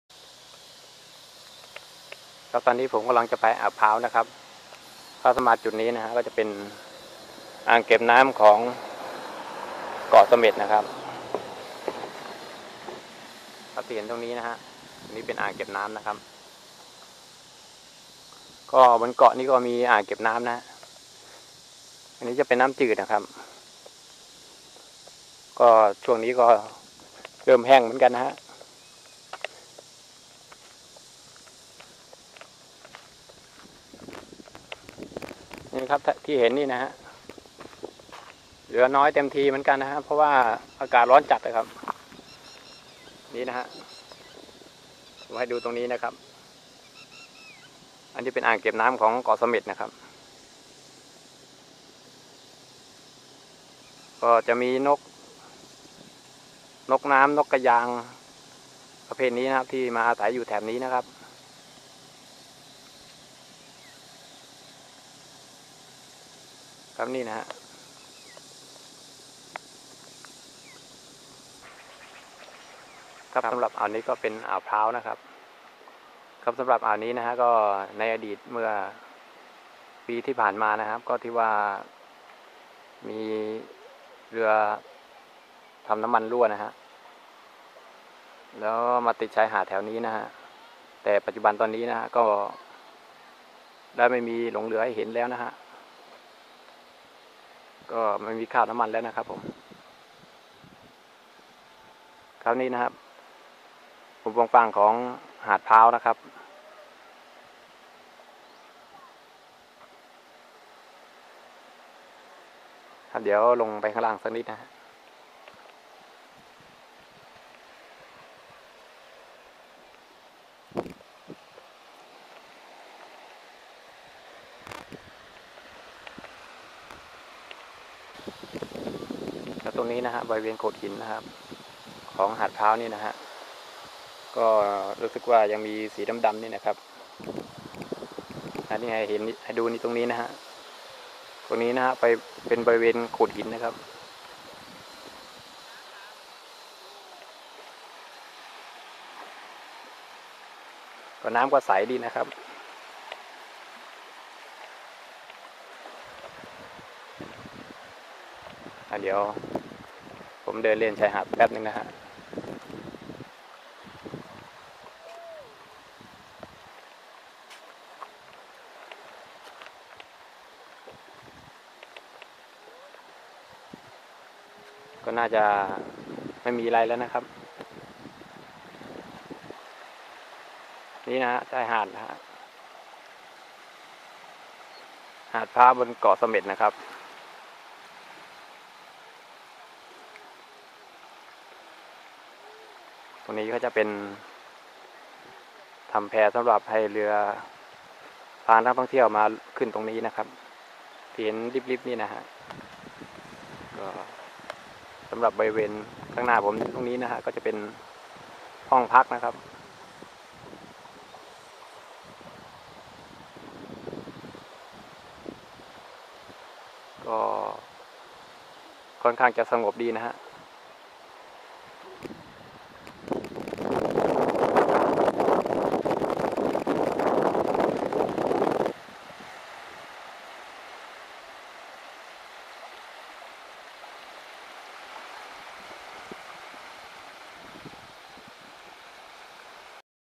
เดินเที่ยวเกาะเสม็ดลงอ่าวพร้าว the bay
เดินเที่ยวชมธรรมชาติบนเกาะเสม็ดในรูปแบบการเดินโดยไม่ต้องขึ้นรถ